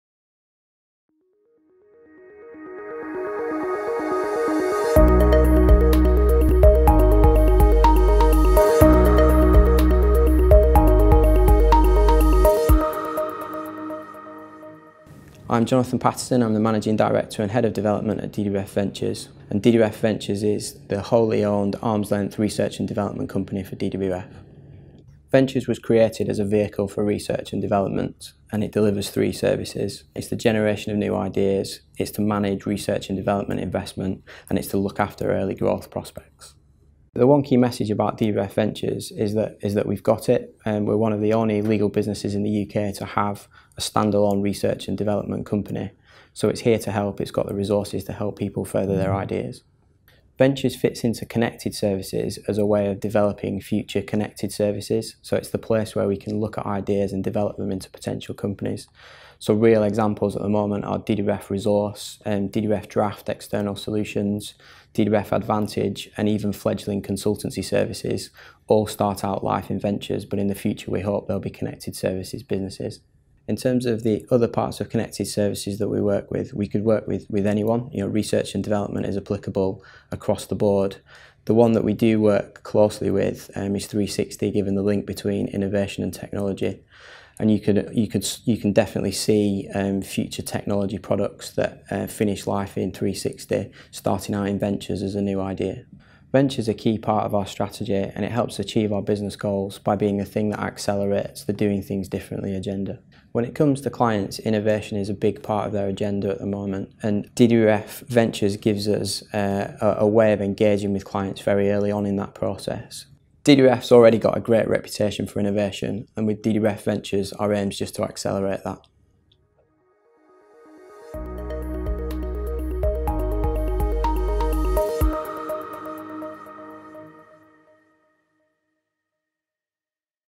Introducing DWF Ventures | Connected Services
DWF Ventures is an innovative new business, designed to help develop new ideas, priorities specific R&D priorities & to nurture early prototype services.